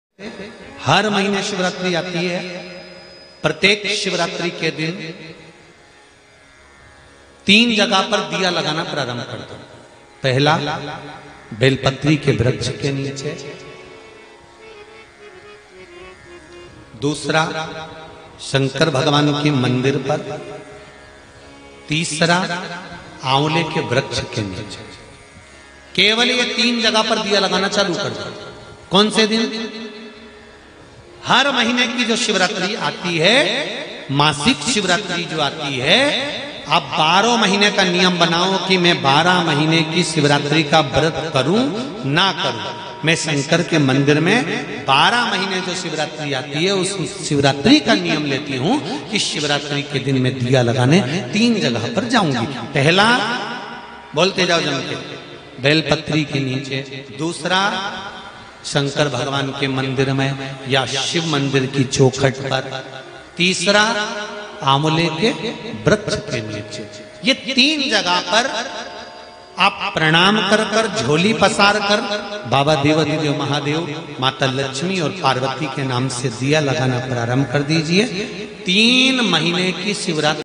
तीन जगह दिए लगाने से होगी सारी मनोकामना पूरी shiv live #2023
Shiv mahashakti

तीनों जगह दिए लगाने से होगी सारी मनोकामना पूरी कौन सी है वह तीन जगह जानी है आज की शिव महापुराण की कथा में हमारे गुरु जी श्री प्रदीप जी मिश्रा जी